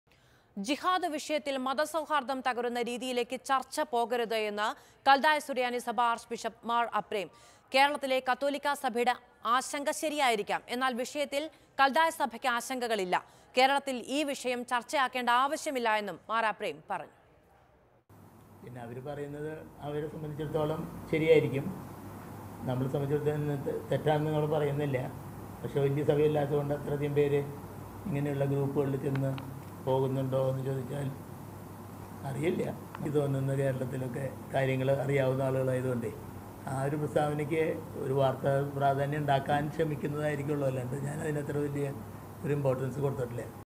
'കേരളത്തിൽ ഈ വിഷയം ചർച്ച ആക്കേണ്ട ആവശ്യമില്ല'; കൽദായ സുറിയാനി സഭാ ആർച്ച് ബിഷപ്പ് | Mathrubhumi News
ജിഹാദ് വിഷയത്തിൽ മതസൗഹാർദം തകരുന്ന രീതിയിലേക്ക് ചർച്ച പോകരുതെന്ന് കൽദായ സുറിയാനി സഭ ആർച്ച് ബിഷപ്പ് മാർ‌ അപ്രേം.കേരളത്തിലെ കത്തോലിക്കാ സഭയുടെ ആശങ്ക ശരിയായിരിക്കാം. എന്നാൽ വിഷയത്തിൽ കൽദായ സഭയ്‍ക്ക് ആശങ്കകളില്ല. കേരളത്തിൽ ഈ വിഷയം ചർച്ച ആക്കേണ്ട ആവശ്യമില്ലെന്നും മാർ അപ്രേം പറഞ്ഞു.

Happy viewing!